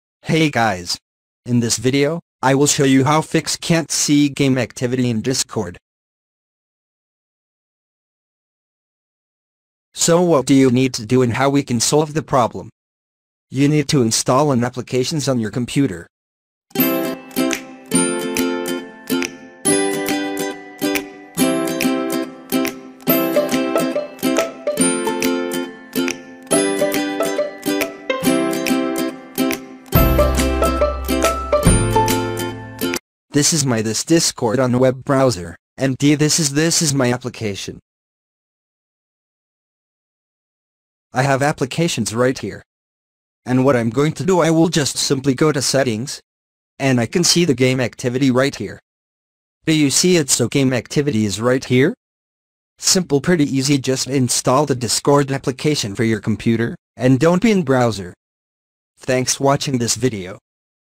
(SOLVED) Can't See Game Activity In Discord | How To Change Now Playing On Discord
In this video, i will show you how fix can't see Game activity in Discord. To solve the problem, you need to install Discord app on your computer.